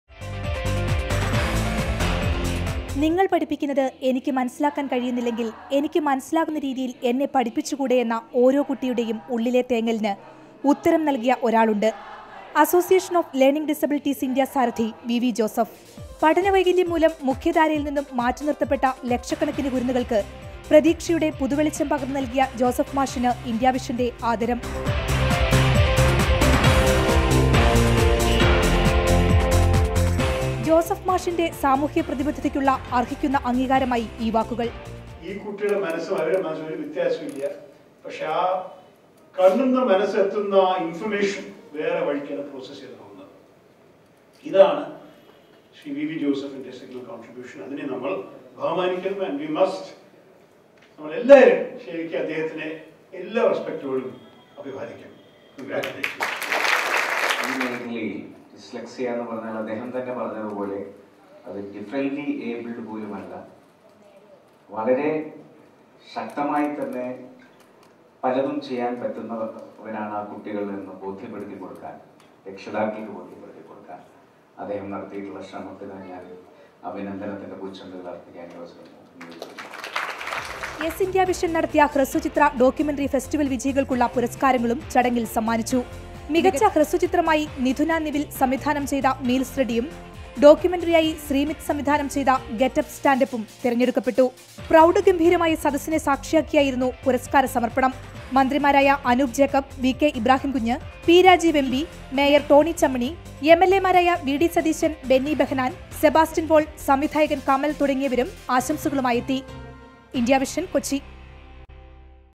Indiavision Person of the Year 2012 award presented
2012-ലെ ഈസ്റ്റേണ്‍- ഇന്ത്യാവിഷന്‍ പേഴ്‌സണ്‍ ഓഫ് ദി ഇയര്‍ പുരസ്‌കാരം അസോസിയേഷന്‍ ഓഫ് ലേണിംഗ് ഡിസബിലിറ്റീസ് ഇന്ത്യാ സാരഥി വി.ഡി. ജോസഫിന് സമ്മാനിച്ചു. കൊച്ചിയില്‍ നടന്ന ചടങ്ങില്‍ കേന്ദ്രമന്ത്രി ശശി തരൂരാണ് പുരസ്‌കാരം നല്‍കിയത്. പഠനവൈകല്യമുള്ള കുട്ടികളെ മുഖ്യധാരയിലേക്ക് കൊണ്ടുവരുന്നതിന് കഴിഞ്ഞ 20 വര്‍ഷമായി നടത്തുന്ന ശ്രമങ്ങളാണ് വിഡി ജോസഫിനെ അവാര്‍ഡിന് അര്‍ഹനാക്കിയത്.